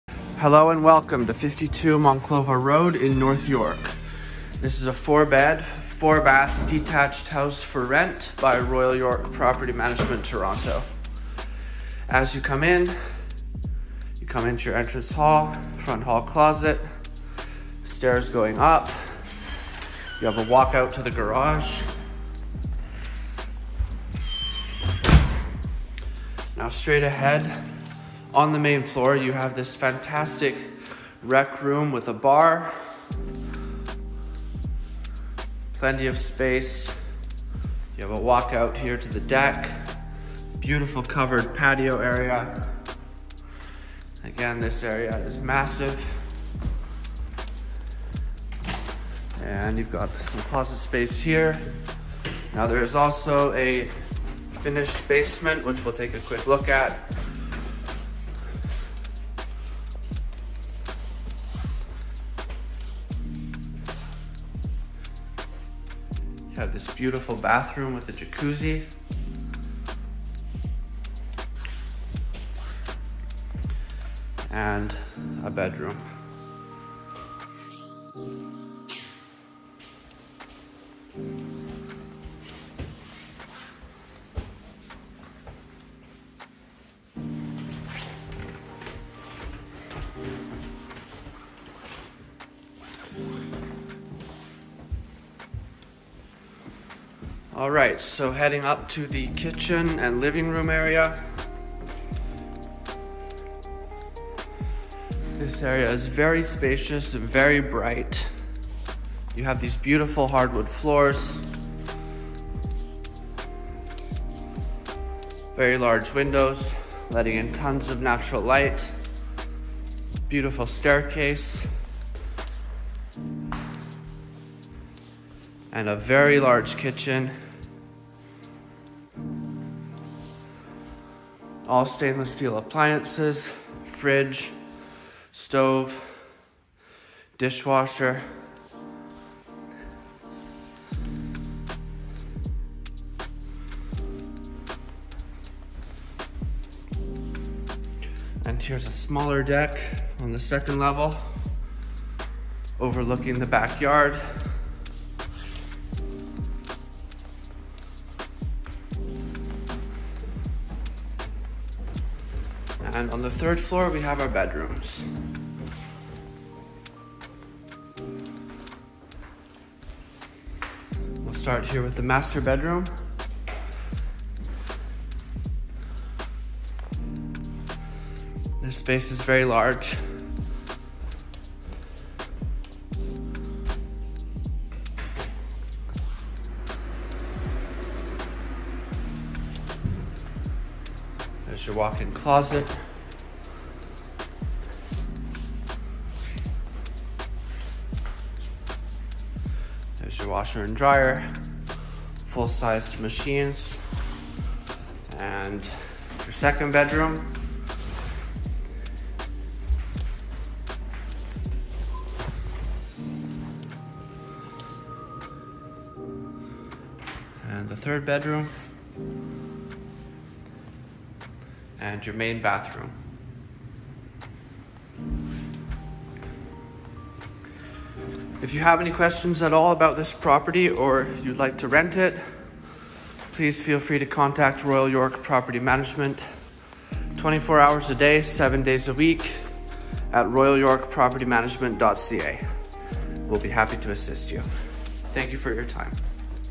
52 Monclova Road, North York, Ontario M3M 0A6
4 Bedrooms | 4 Bathrooms, Upgraded Kitchen, Backsplash, Stainless Steel Appliances, Formica Countertops, Dishwasher, Microwave, Ensuite Laundry, Personal Thermostat, Hardwood and Vinyl Floors, Walk-out to Garage, Private Balcony, Tons of Natural Light.

Close to Highway 401 and 400, Jane Street at Giltspur Drive, Chalkfarm Public School, Sheppard Public School, Belmar Park, Downsview Dells Park, Parc Downsview Park, North York Sheridan Mall, Downsview Shopping Center, Canadian Air and Space Museum

52 Monclova Road, North York, Ontario M3M 0A6
Monclova Road and Exbury Road

Located in a Quiet Area!!

$3,900 (Three Parking Spots Included)

UNIT AMENITIES:
*4 Bedrooms | 4 Bathrooms
*Upgraded Kitchen
*Upgraded Backsplash
*Upgraded Bathroom
*Stainless Steel Appliances
*Formica Countertops
*Dishwasher
*Microwave
*Ensuite Laundry
*Personal Thermostat
*Hardwood and Vinyl Floors
*Walk-out to Garage
*Private Balcony

Available Immediately!!